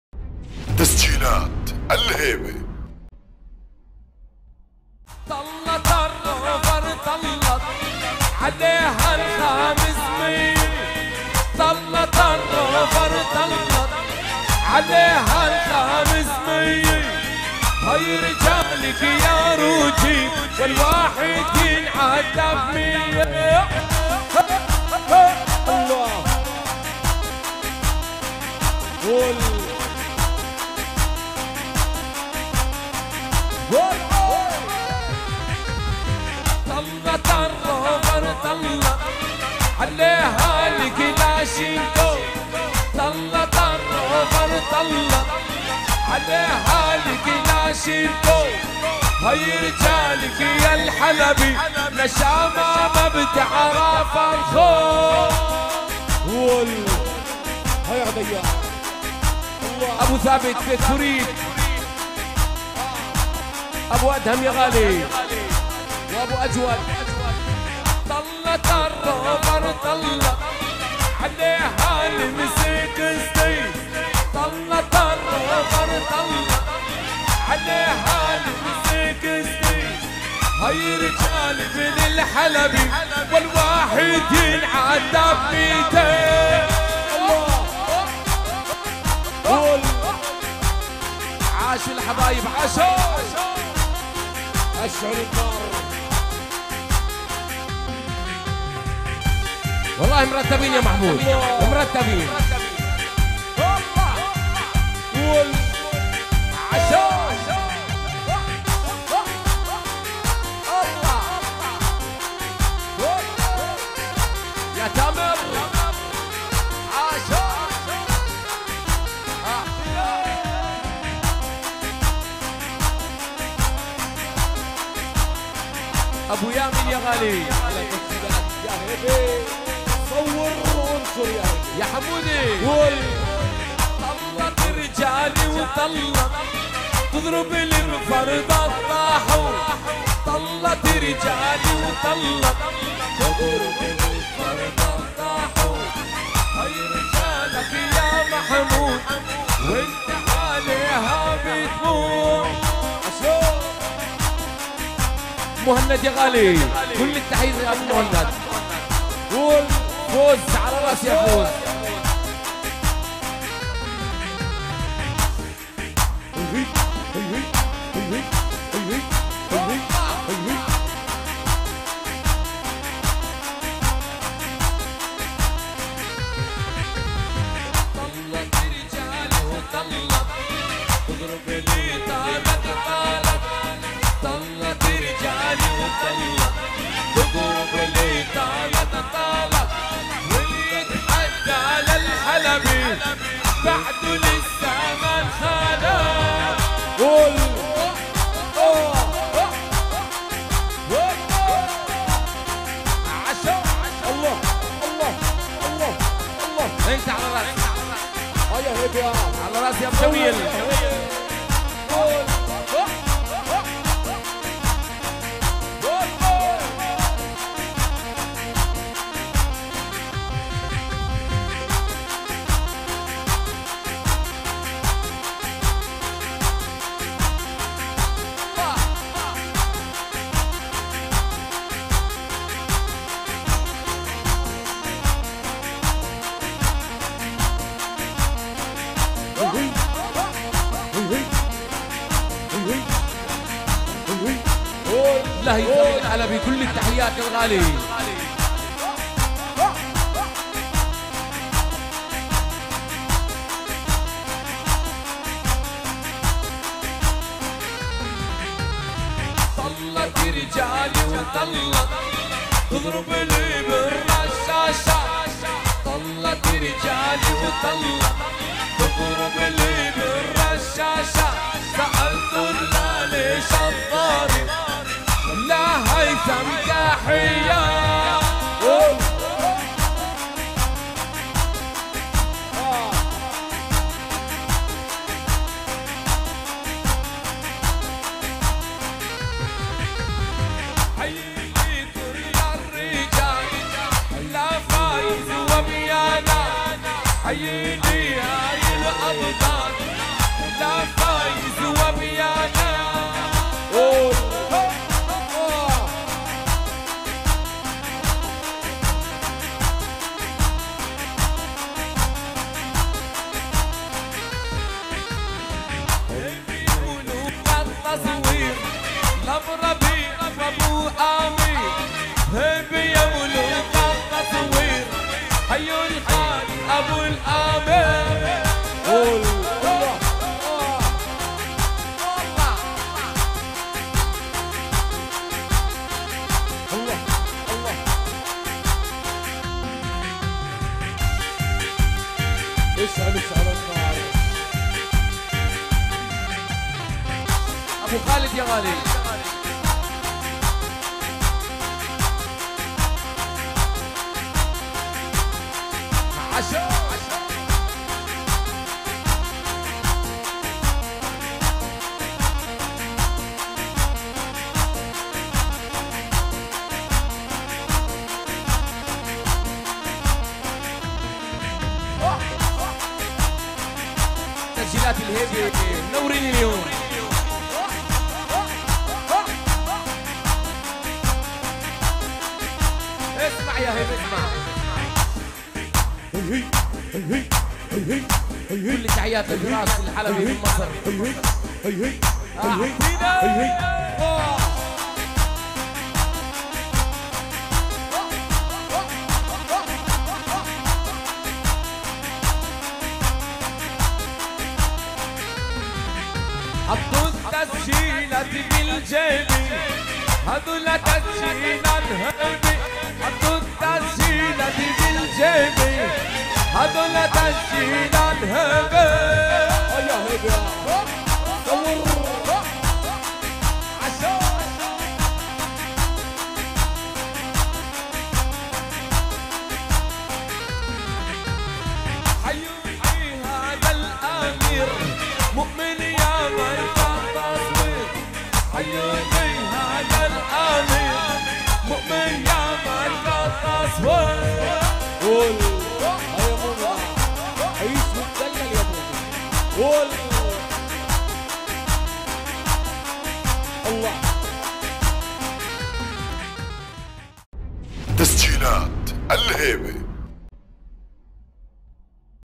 حسنين المصري يتسبب بحرب في روجيب بسبب دحية في غداء العريس هادي الحلبي
مع تحيات تسجيلات الهيبة
للحجز والاستفسار الاتصال على: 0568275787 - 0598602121